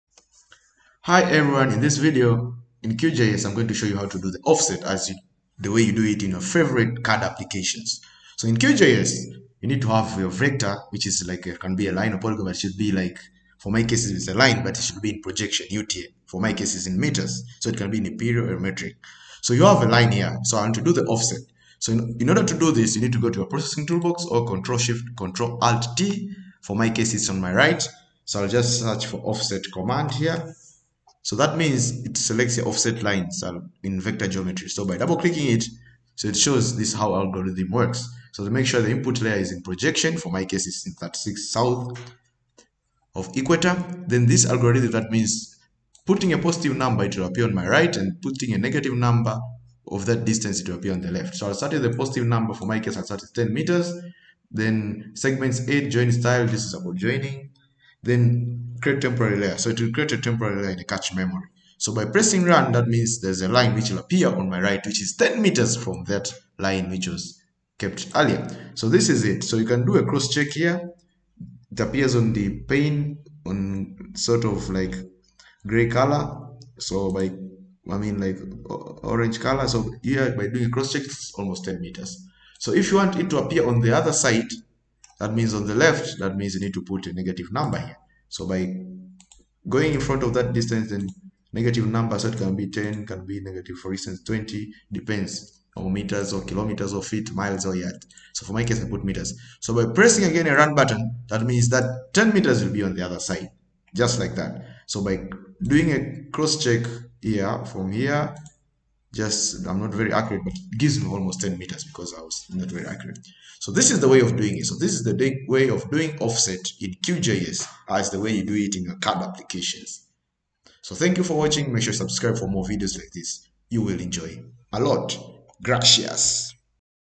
How to offset line in QGIS
The video shows how to offset lines in both right hand and left hand side.